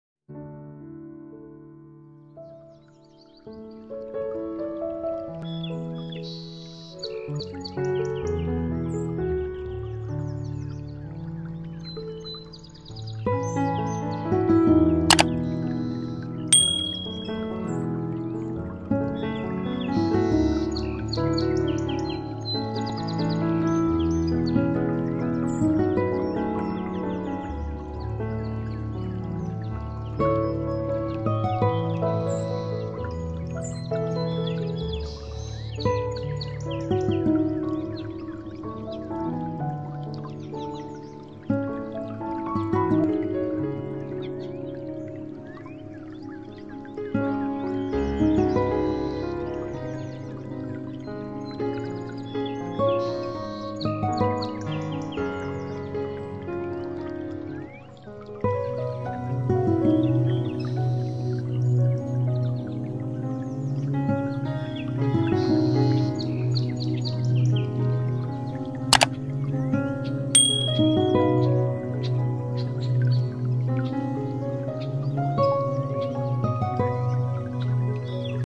Most Beautiful Birds parrots relexing baground music 2021 HABIB ULLAH
In this video you will see the most beautiful birds and relaxing baground music enjoy